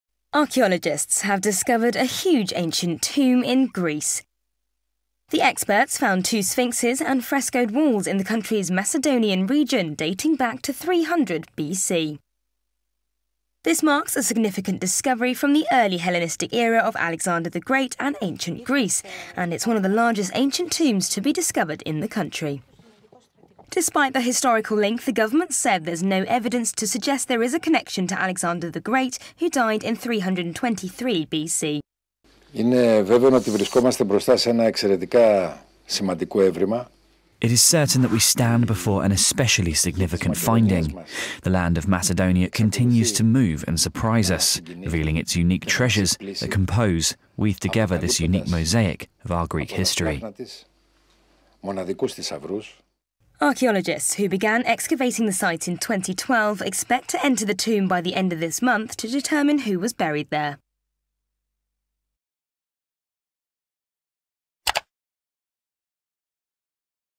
Archaeologists discover huge ancient tomb in Greece
Archaeologists have discovered a huge ancient tomb in Greece dating back to 300BC. The experts found two sphinxes and frescoed walls in the country's Macedonian region. This marks a significant discovery from the early Hellenistic era of Alexander the Great and ancient Greece and is one of the largest ancient tombs to be discovered in the country. Report by Sarah Kerr.